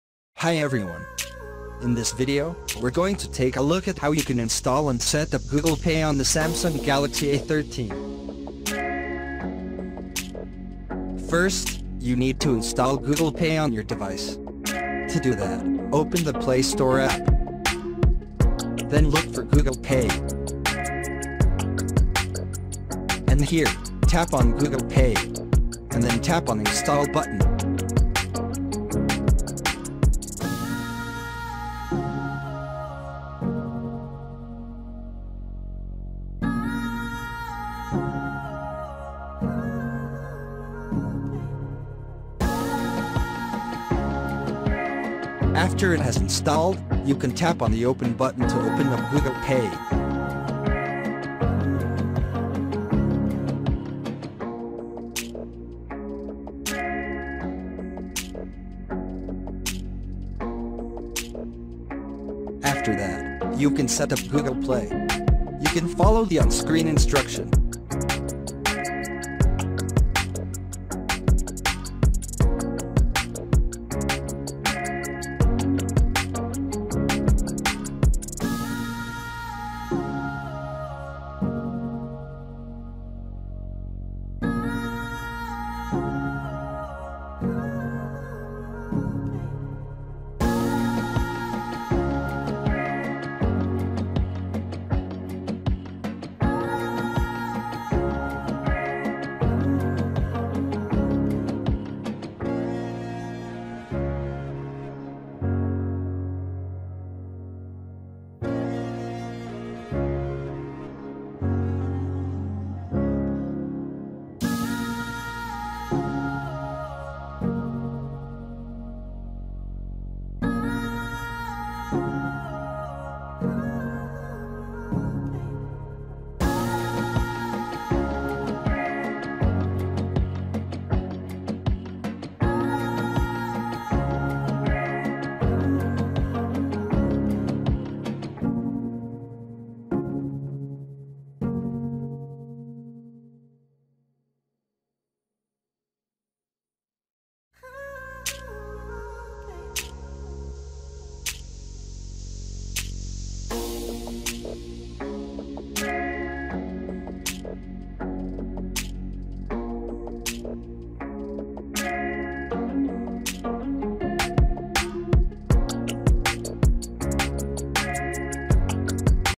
Samsung Galaxy A13: How to Install & Setup Google Pay
Learn how you can install & Setup Google Pay on Samsung Galaxy A13.